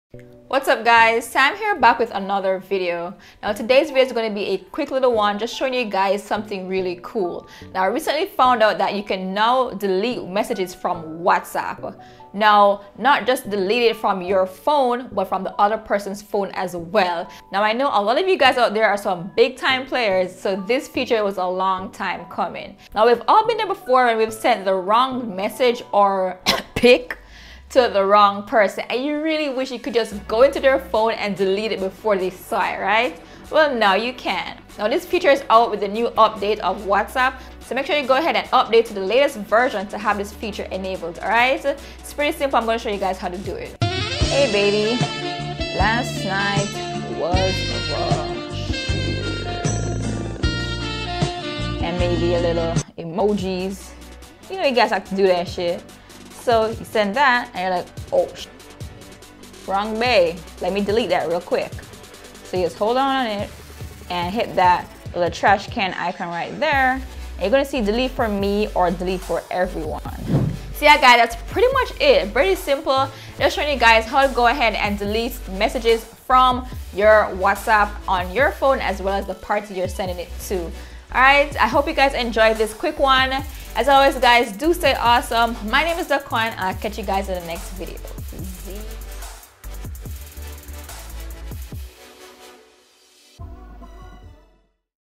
How to UN-SEND/DELETE messages from Whatsapp!!
Both parties needs to have the updated version of Whatsapp
It only works within 7 minutes of sending the message The recipient must not have read /opened the message as yet

😊Periscope: Dakoin